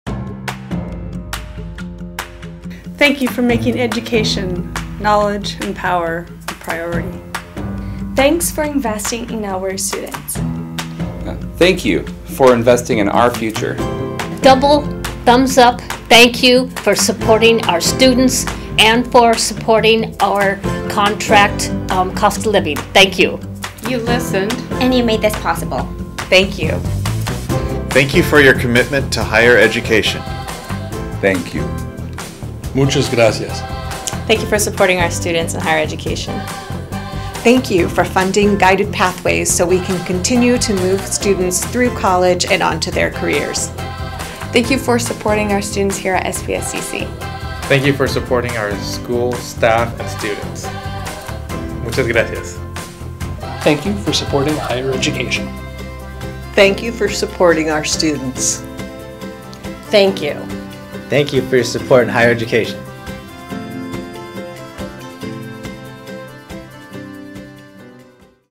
Thank You, Legislators
SPSCC faculty, staff, and students want to thank the Washington State Legislature for funding our community college system. Thank you for investing in the success of our students and the success of our communities.